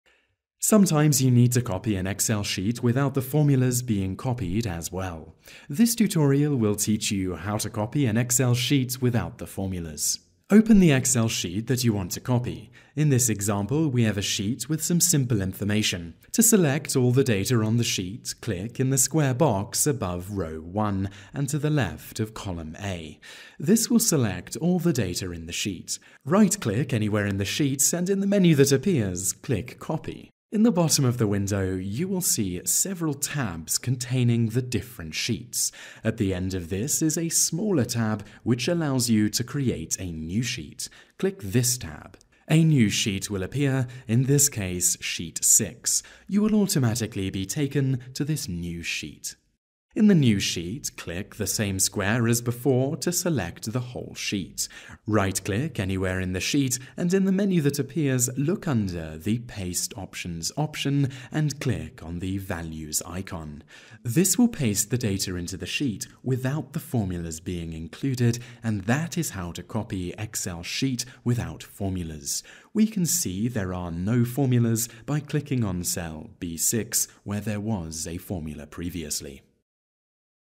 How to Copy Excel 2010 sheet without formulas
This tutorial will show you how to copy an excel sheet without copying the formulas as well

Sometimes you need to copy a sheet without the formulas being copied as well. This tutorial will teach you how to copy excel sheet without formulas.

Step # 1 -- Choosing the Source Sheet and Selecting All

Open the Excel sheet that you want to copy. In this example we have a sheet with some simple information. To select all of the data on the sheet click in the square box above row "1" and to the left of column "A". This will select all of the data in the sheet. Right click anywhere in the sheet and in the menu that appears click "Copy".

Step # 2 -- Create a Destination Sheet

In the bottom of the window you will see several tabs containing the different sheets. At the end of this is a smaller tab which allows you to create a new sheet, click this tab. A new sheet will appear, in this case "Sheet6". You will automatically be taken to this new sheet.

Step # 3 -- Pasting Just the Values

In the new sheet click the same square as before to select the whole sheet. Right click anywhere on the selected sheet and in the menu that appears look under the "Paste Options" option and click on the "Values" icon. This will paste the data into the sheet without the formulas included and that is how to copy excel sheet without formulas. We can see there are no formulas by clicking on cell "B6" where there was a formula previously.